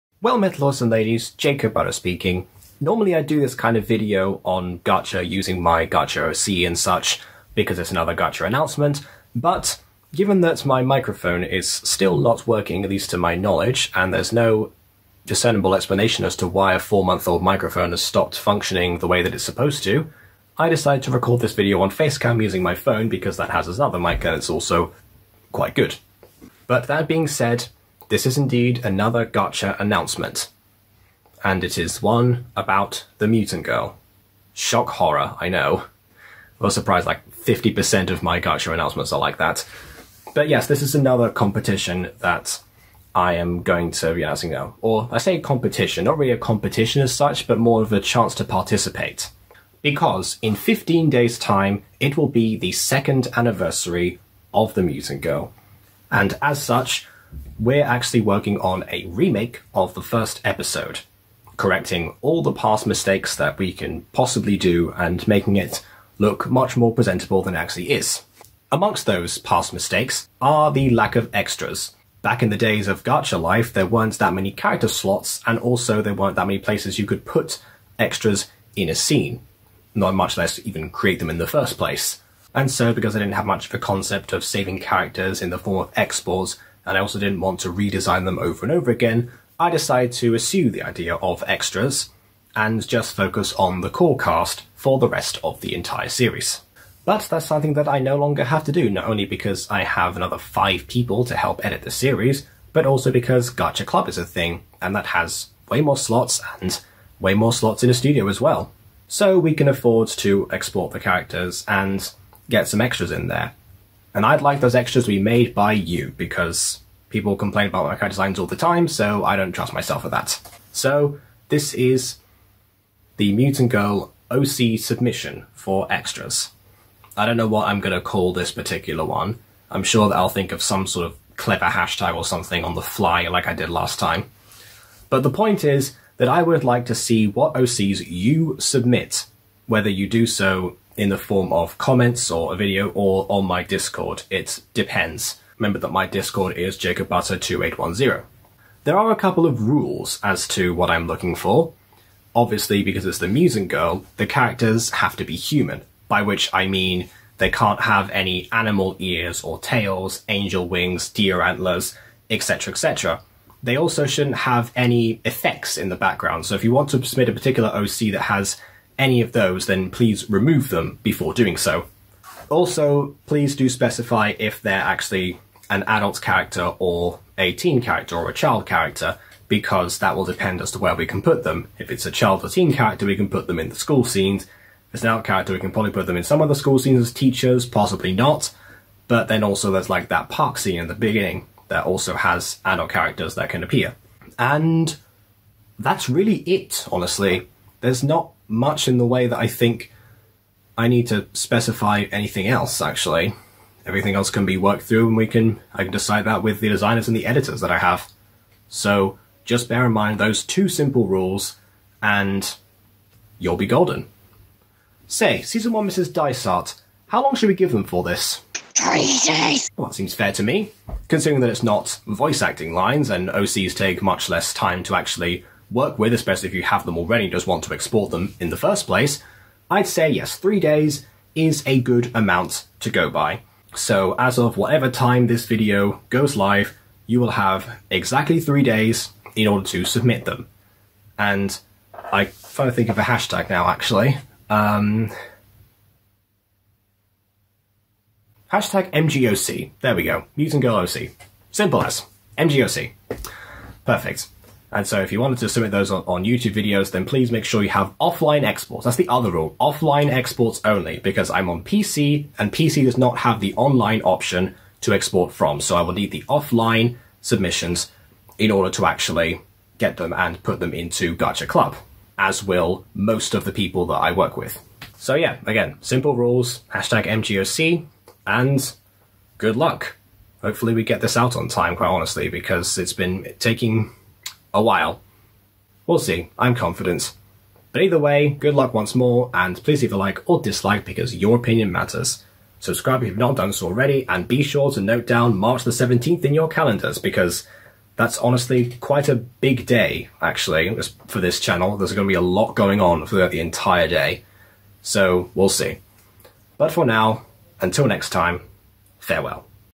OC Contest [Closed] for The Mutant Girl
Reminder of the Rules:
1. OC must be human.
2. Must specify OC age.
3. Offline Exports only.

Deadline: February 6th 2021 at 3:00am GMT

Submit via YouTube with the MGOC, in the comments of THIS VIDEO or on my Discord (Jacob Butter#2810). Good luck.